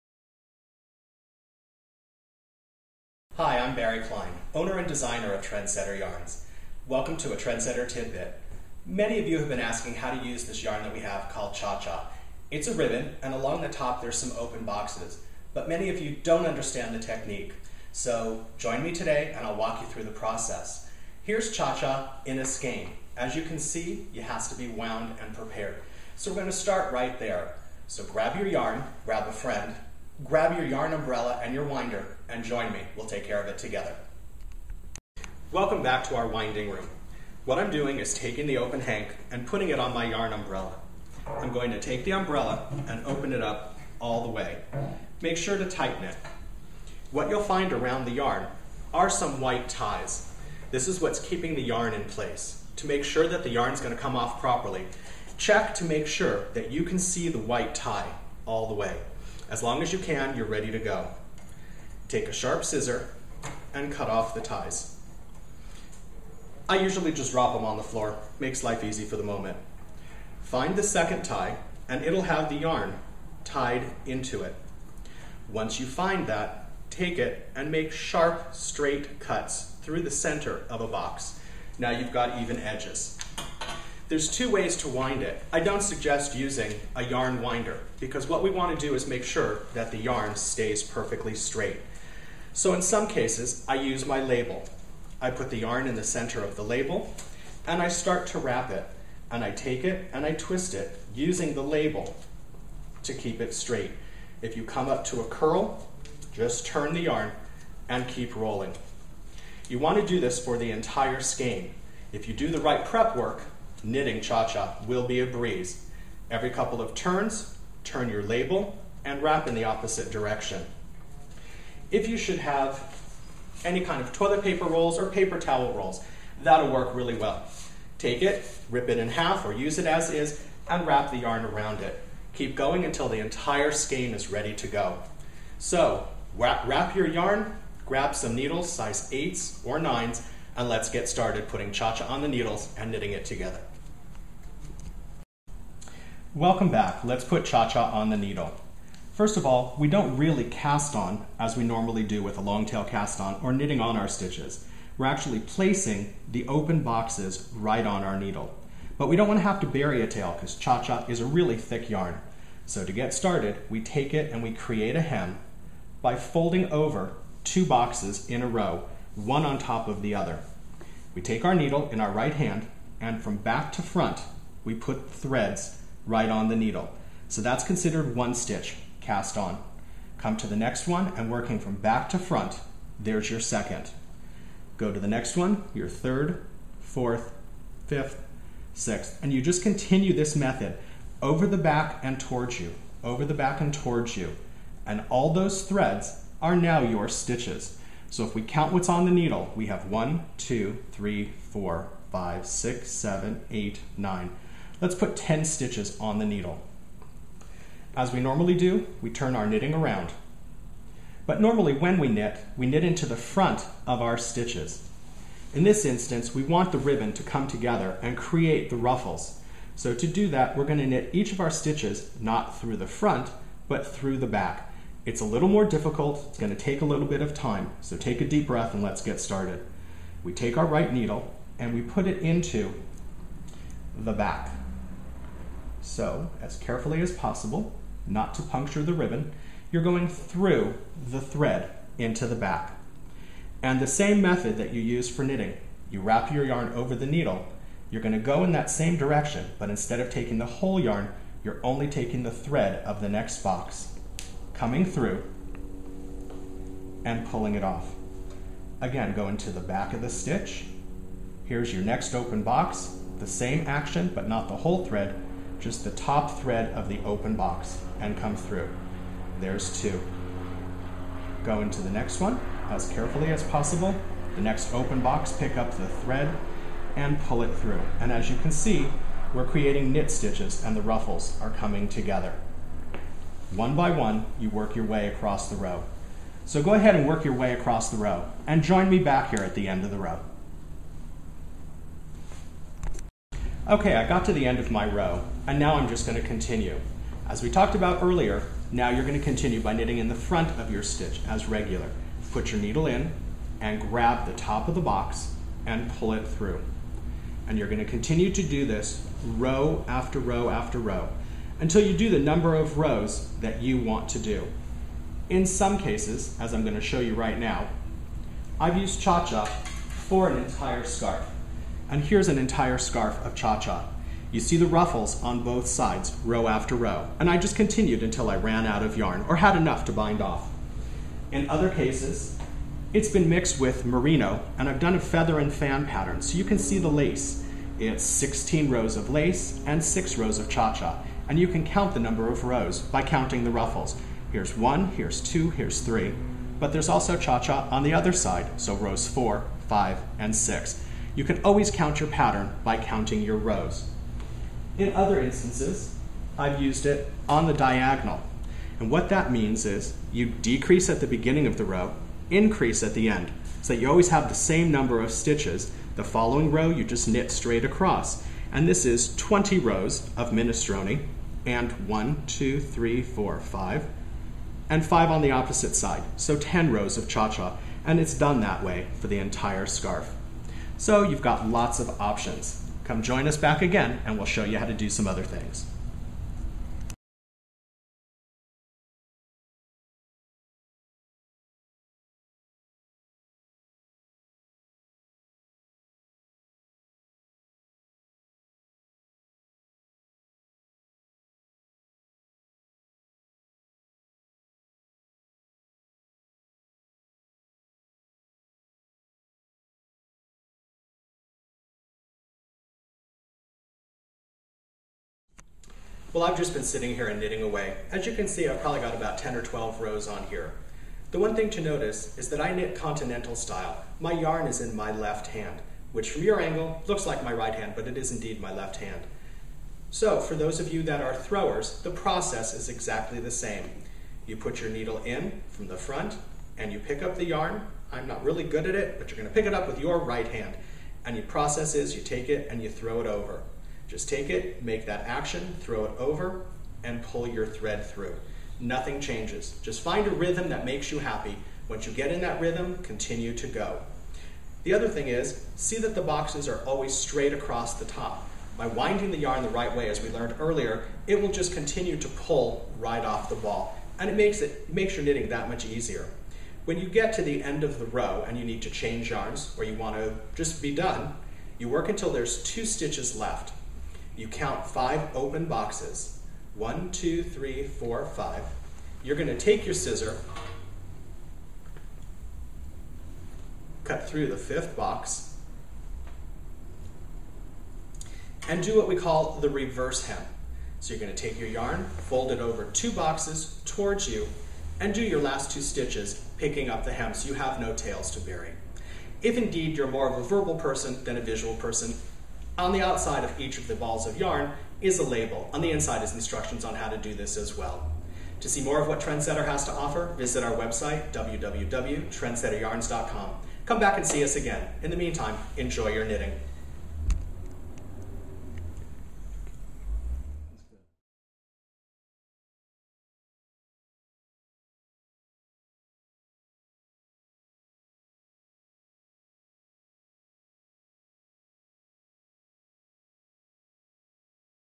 Trendsetter Tidbit - Knitting Cha Cha Yarn
Enjoy as owner and designer of Trendsetter Yarns, Barry Klein, walks you through knitting the tricky, but rewarding, cha cha yarn!  Enjoy!